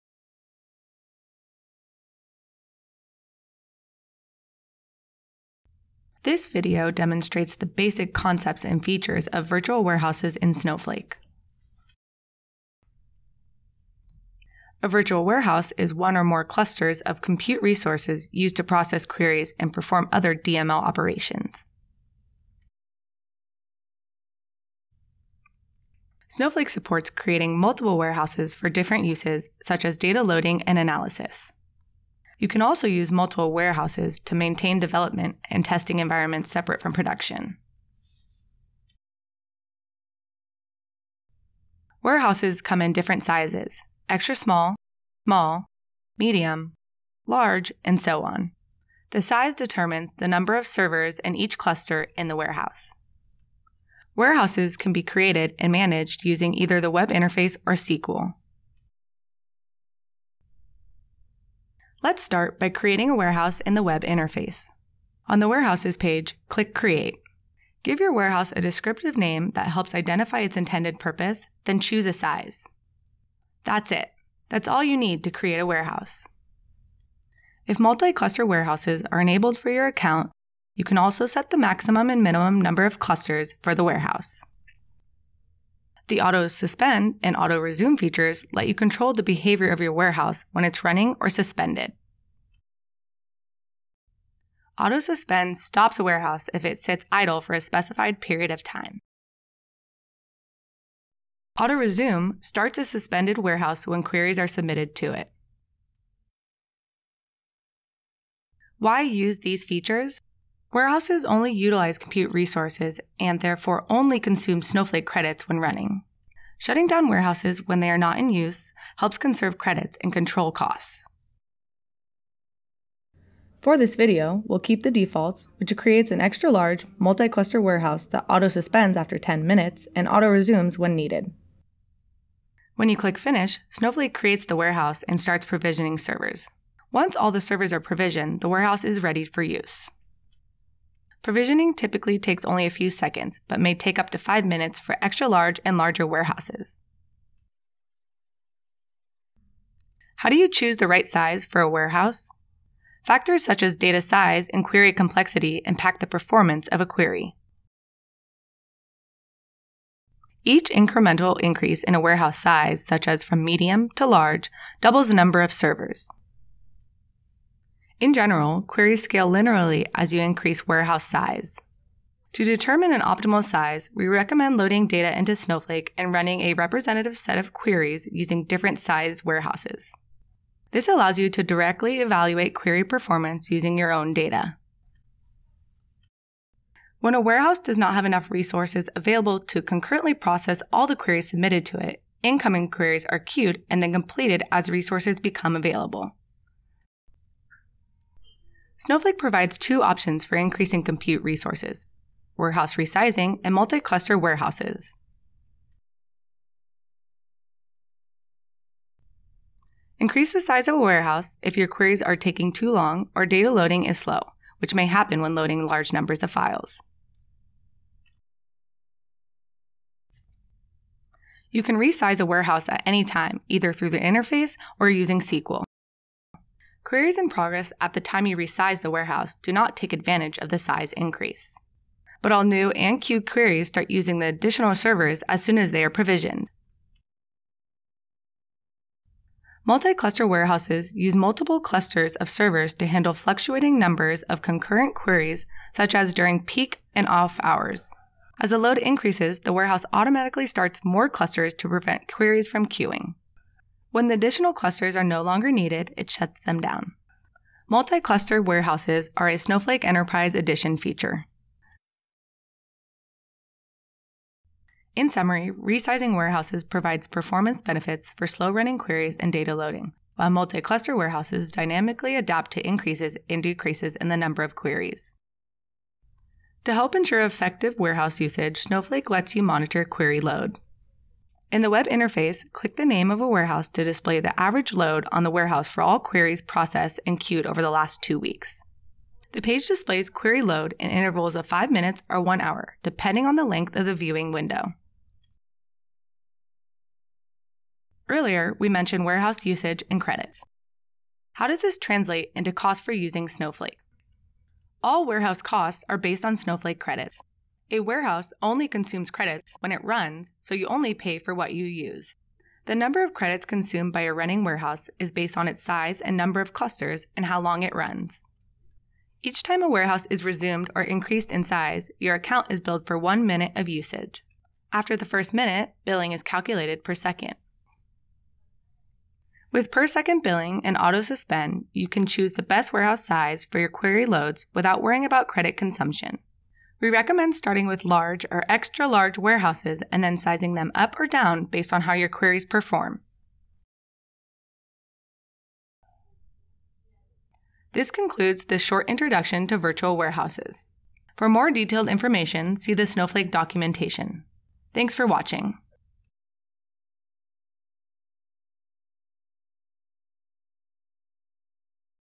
Getting Started - Introduction to Snowflake Virtual Warehouses
This video has been moved to our new Snowflake Developers Youtube Channel - check out the video here:

This video provides a conceptual overview of virtual warehouses in Snowflake and illustrates how to use them to perform queries and load data.

A virtual warehouse is one or more clusters of compute resources used to process queries and perform other DML operations. Snowflake supports creating multiple warehouses for different uses such as data loading and analysis, and testing environments separate from production.

This is a new version of a previous video. The content is similar, but has been updated to reflect the latest Snowflake functionality.